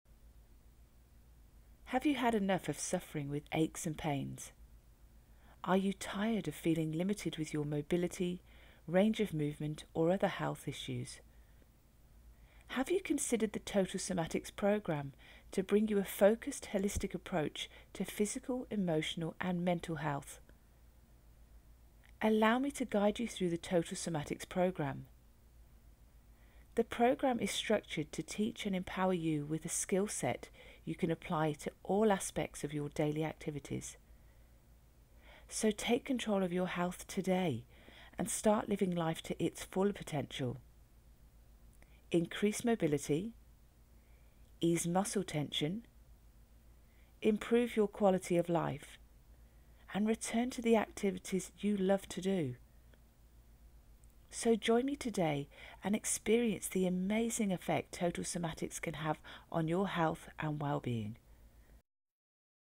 Introduction To Movement Therapy
Relax Tight Muscles
Take Pressure Off Your Joints
Switch muscles on and off.
Improve mobility.